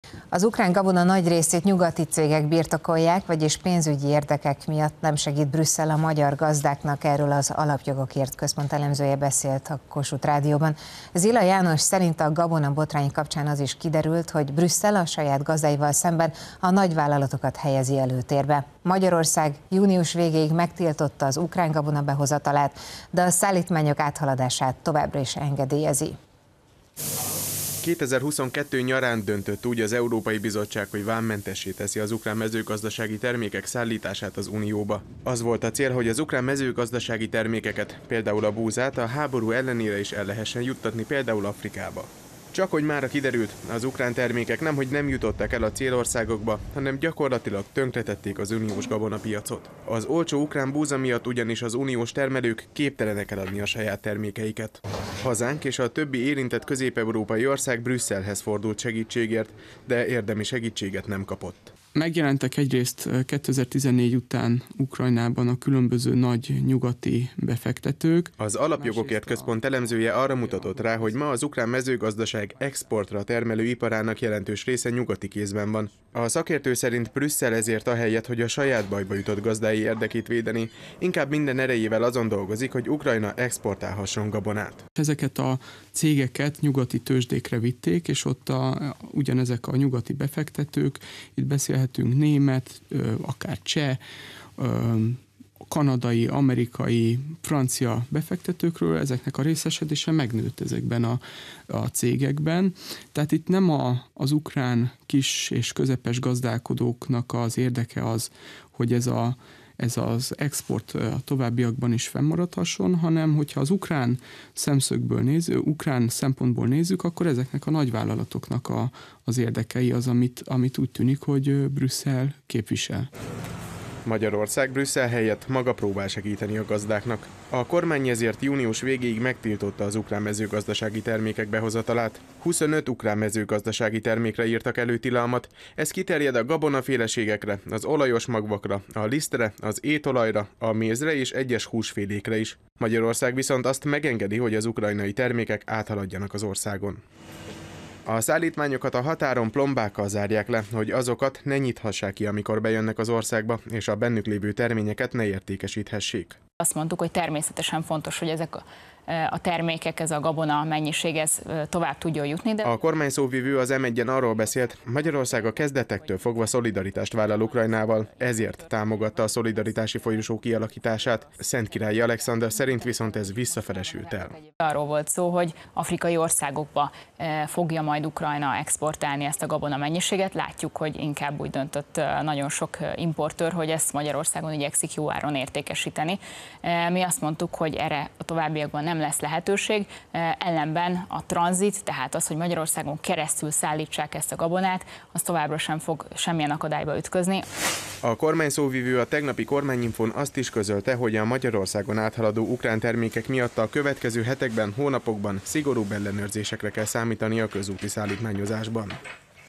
Elemző: az ukrán gabona nagy részét nyugati cégek birtokolják
Az ukrán gabona nagy részét nyugati cégek birtokolják, vagyis pénzügyi érdekek miatt nem segít Brüsszel a magyar gazdának - erről az alapjogokért központ elemzője beszélt a Kossuth Rádióban. Zila János szerint a gabonabotrány kapcsán az is kiderült, hogy Brüsszel - a saját gazdáival szemben - a nagyvállalatokat helyezi előtérbe. Magyarország június végéig megtiltotta az ukrán gabona behozatalát, de a szállítmányok áthaladását továbbra is engedélyezi.